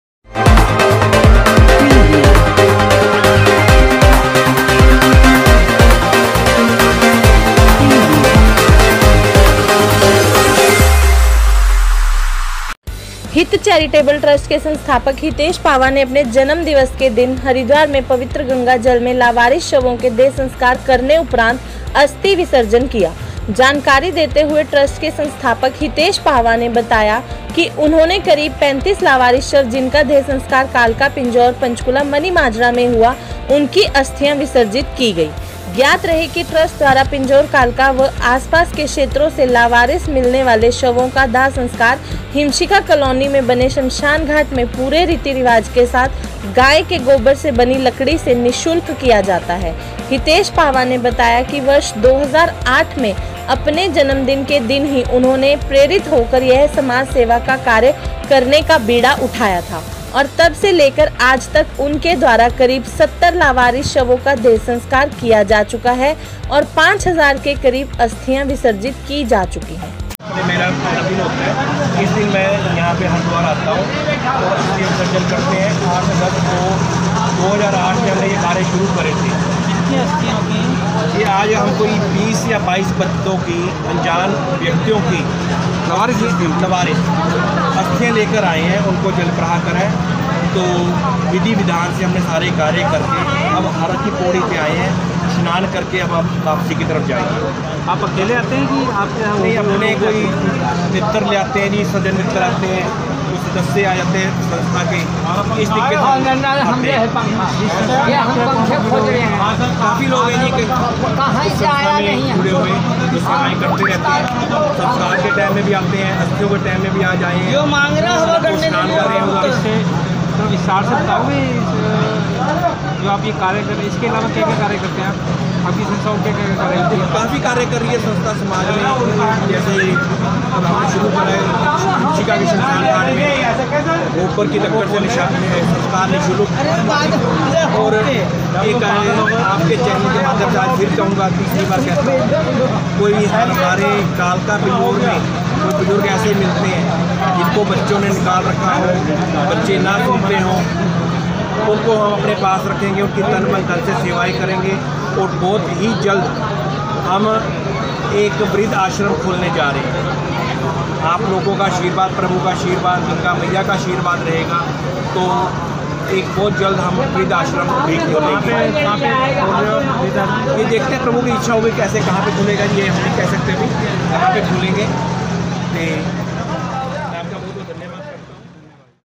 Like comment and share subscribe
हितेश पाहवा ने अपने जन्मदिवस के उपलक्ष्य में मां गंगा के चरणों में लावारिस शवों की अस्थियां विसर्जित की।
9-8-2020